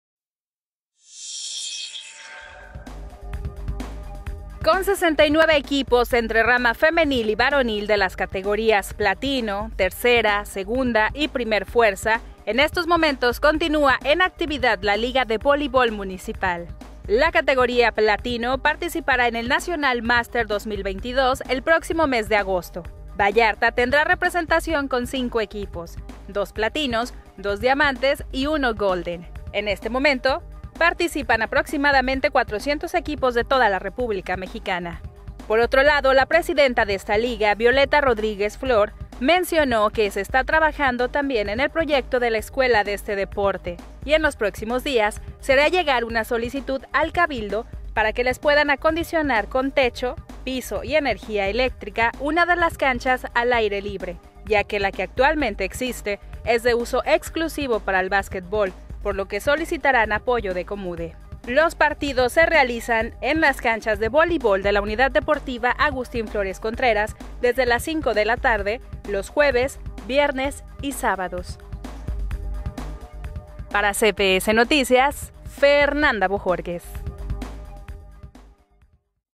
Continúa la Liga de Voleibol municipal | CPS Noticias Puerto Vallarta
Con 69 equipos entre rama femenil y varonil de las categorías,  platino,  3era, 2da y 1er fuerza en estos momentos, continúa en actividad la liga de voleibol municipal.